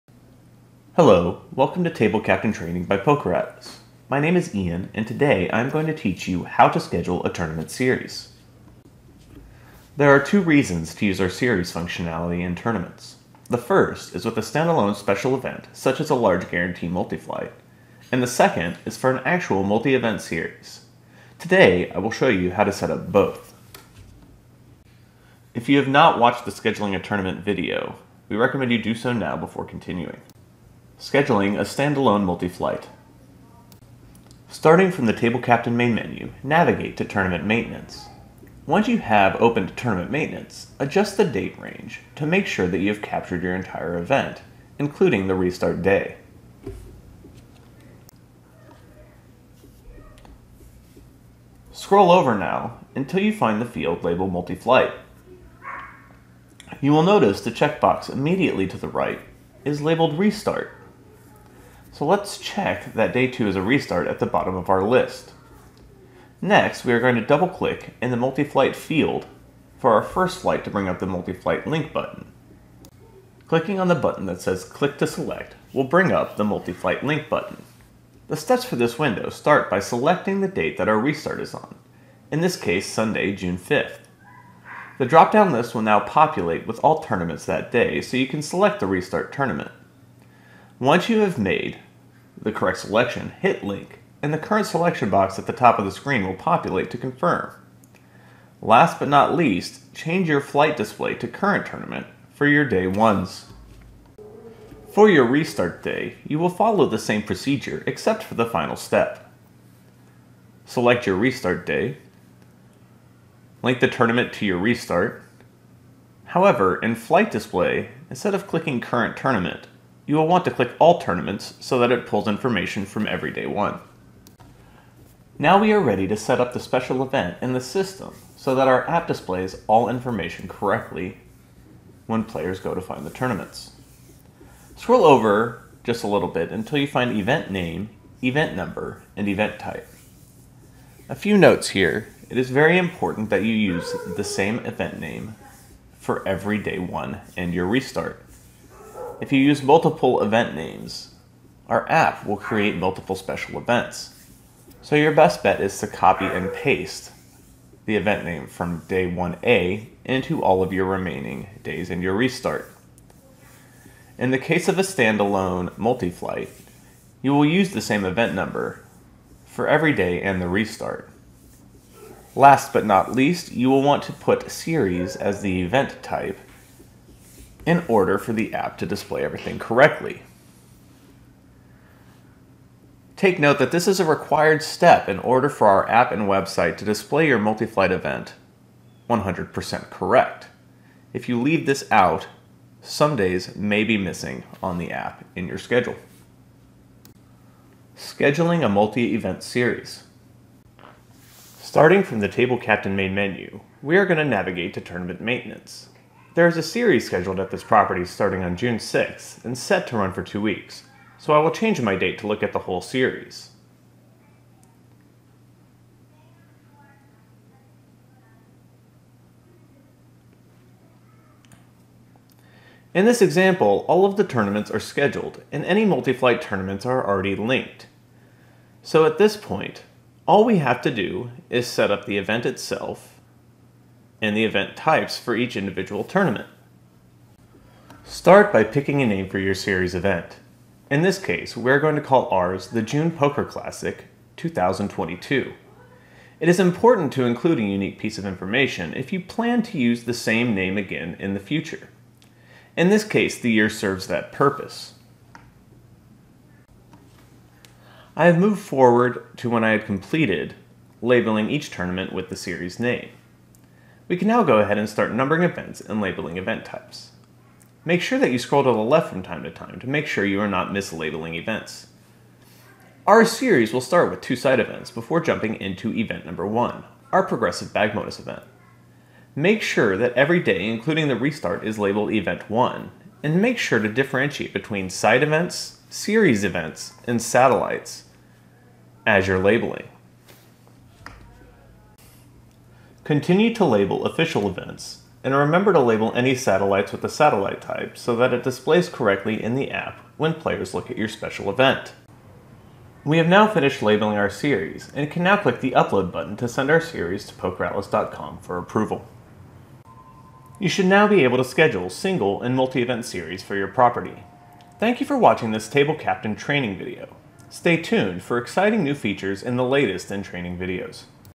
Scheduling a Tournament Series & Multi Flight Linking
In this video from PokerAtlas TableCaptain, learn how to schedule a singular multi flight event, as well as a multi event series. Take your tournaments to the next level with PokerAtlas TableCaptain.